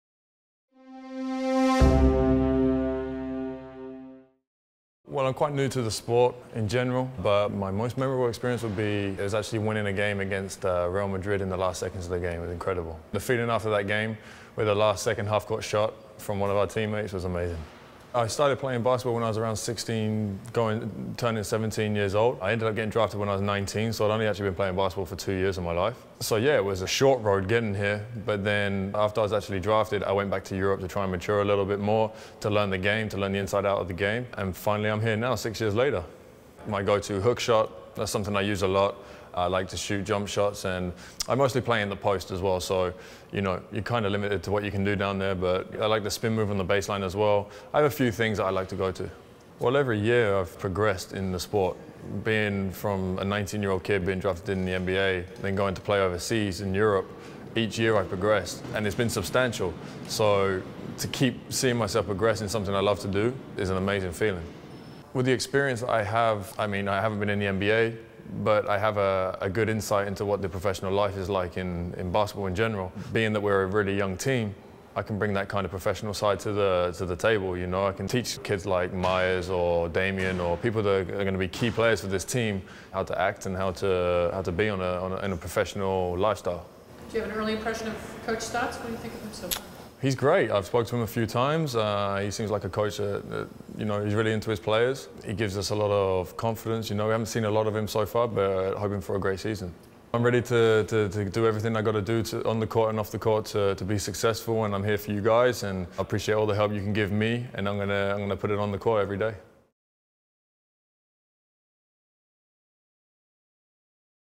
Meet the Trail Blazers 2012: power forward Joel Freeland
The Blazers' new power forward talks about his experience playing in Europe and how he hopes it can benefit both himself and other rookies on the team.